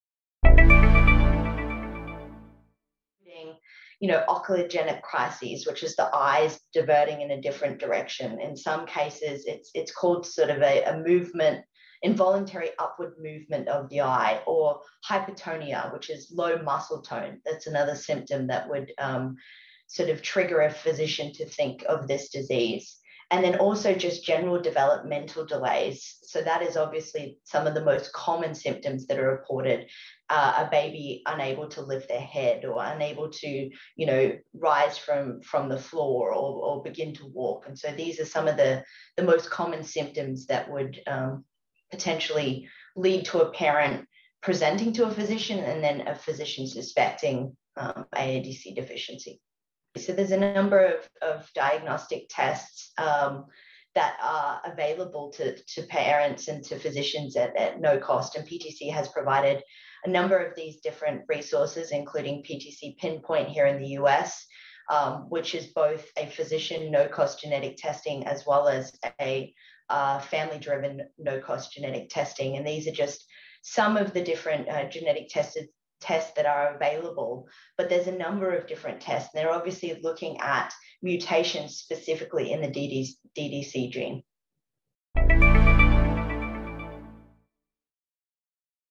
When to Suspect AADC Deficiency in a Baby
Kylie O’Keefe, Senior Vice President, Head of Global Commercial and Corporate Strategy at PTC Therapeutics, describes early signs and symptoms of aromatic L-amino acid decarboxylase (AADC) deficiency.

AADC deficiency is a rare, genetic disorder due to a defect in the dopa decarboxylase (DDC) gene that reduces the synthesis of dopamine, norepinephrine, epinephrine, and melatonin.

As Ms. O’Keefe explains, two common signs of AADC deficiency are oculogyric crises, which is prolonged involuntary upward deviation of the eyes, and hypotonia. General developmental delays may also inspire a physician to test for AADC deficiency.

There are a number of diagnostic tests for AADC deficiency, as Ms. O’Keefe says. PTC Pinpoint is one such test available in the U.S. and no-cost to physicians and families.